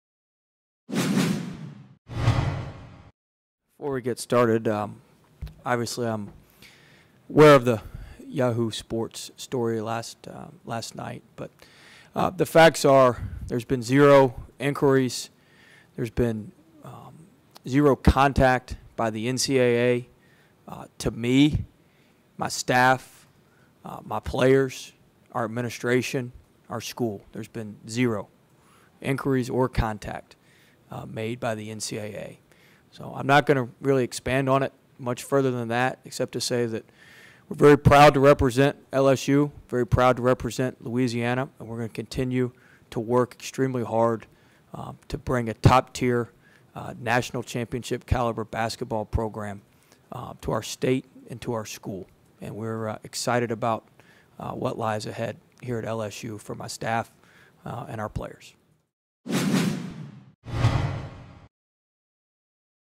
LSU HC Will Wade discusses NCAA probe into his recruiting
"There's been zero inquiries, there's been zero contact by the NCAA, to me, my staff, my players, our administration, our school." -LSU's Head Coach's comments on NCAA probe.